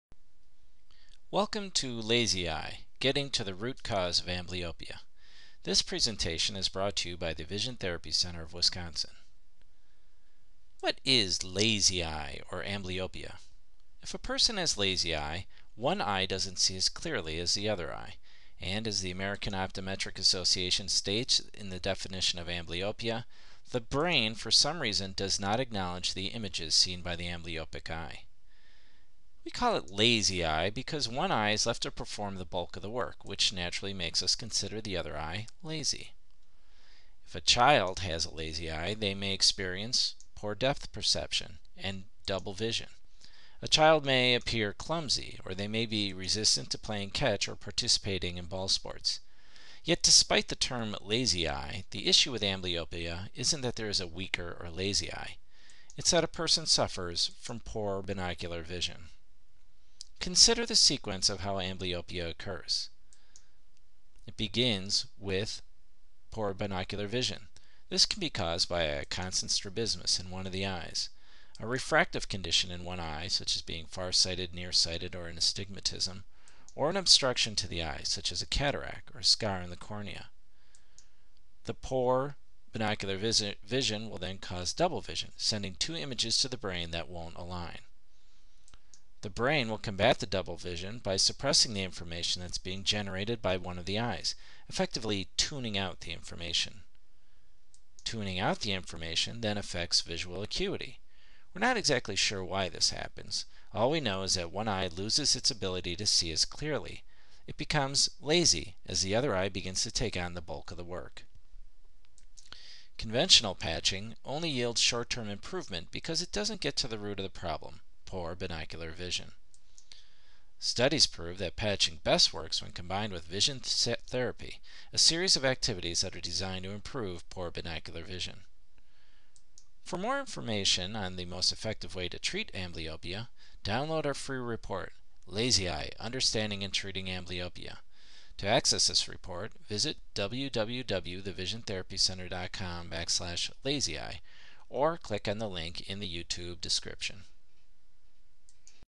What is Lazy Eye?
Fixing a lazy eye can be done.  You can overcome amblyopia.  But to do it, you are going to have to do more than just treat the symptoms.  You're going to have to get to the root cause of amblyopia.
This involves more than just patching.  It requires understanding and getting to the root of the problem -- poor binocular vision.  In the short video below, we explain why binocular vision is the root cause of amblyopia.  Once you've watched it, keep on reading to understand how to get to the treatment.